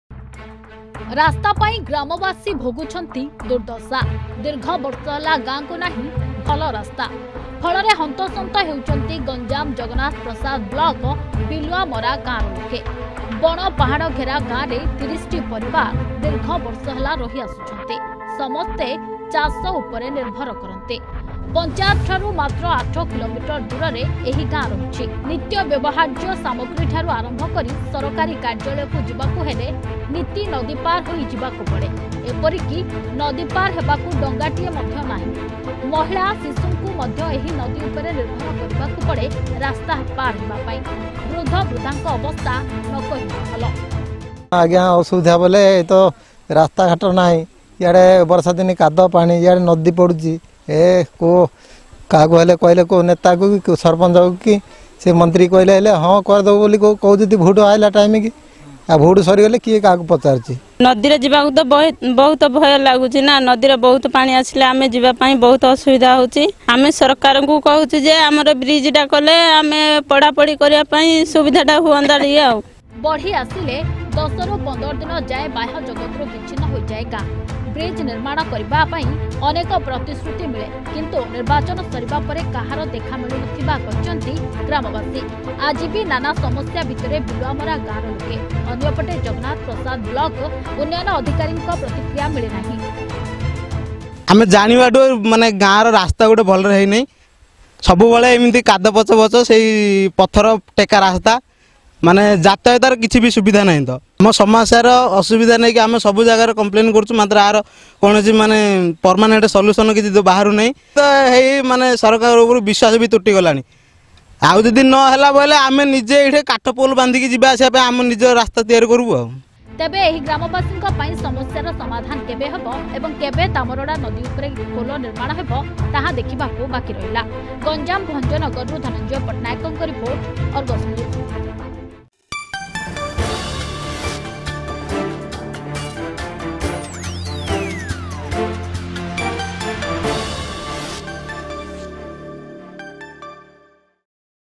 ରାସ୍ତା ପାଇଁ ଗ୍ରାମବାସୀ ଭୋଗୁଛନ୍ତି ଦୁର୍ଦ୍ଦଶା
ସ୍ୱାଧୀନତାର ସାତ ଦଶନ୍ଧି ପରେବି ଲୋକଙ୍କ ପାଖରେ ପହଂଚି ପାରିନି ମୌଳିକ ସୁବିଧା । ଶିକ୍ଷା ସ୍ୱାସ୍ଥ୍ୟ ଗମନାଗମନ ସବୁ କିଛି ଅପହଞ୍ଚ । ଆଜିବି ଗାଁକୁ ଯାତାୟାତ କରିବା ପାଇଁ ପକ୍କା ରାସ୍ତା ଟିଏ ନାହିଁ । ଗାଁ ଲୋକଙ୍କ ପାଇଁ କାଳ ସାଜିଛି ତାମରଡ଼ା ନଦୀ.